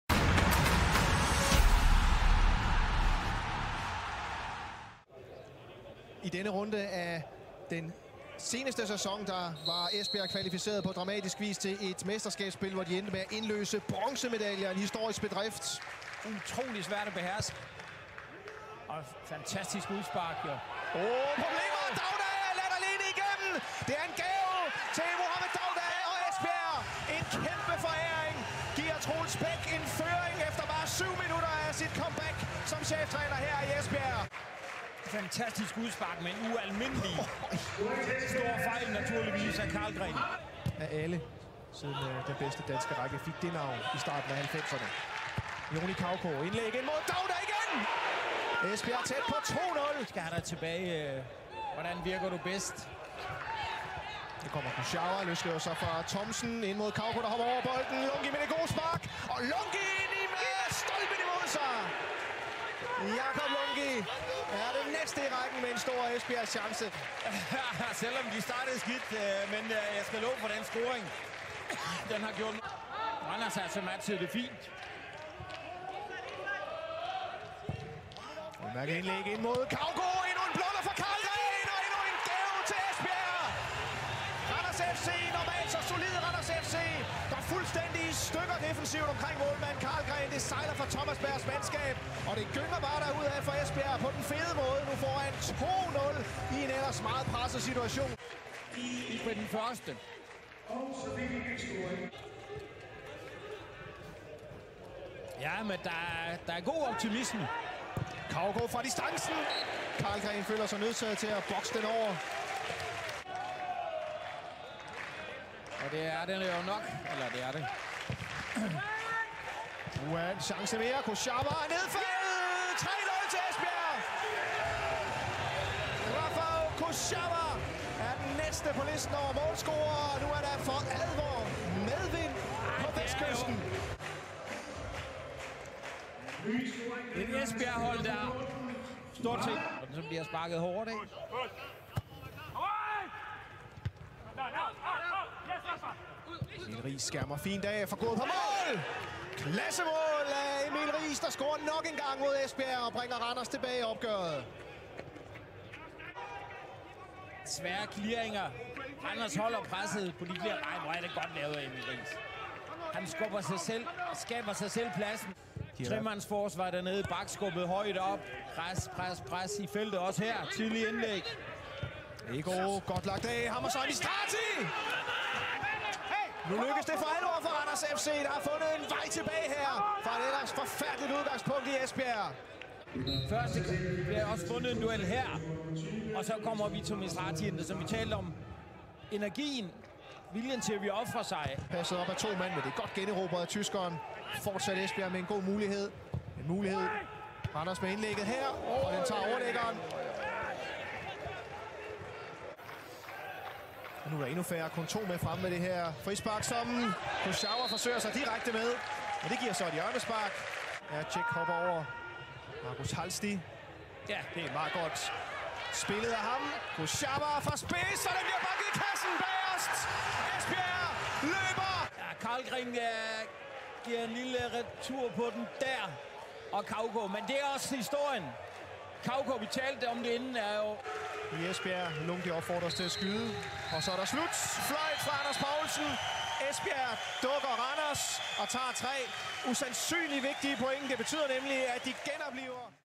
Esbjerg fB - Randers FC 4-2 (12-6-2020)
Randers FC-spillerne var igen tæt på at lave et comeback efter at have været bagud 3-0 i det vestjyske. Det endte dog med et 4-2-nederlag, da Randers FC indledte gruppespillet i Esbjerg.

Emil Riis og Vito Mistrati noterede sig for de kronjyske scoringer.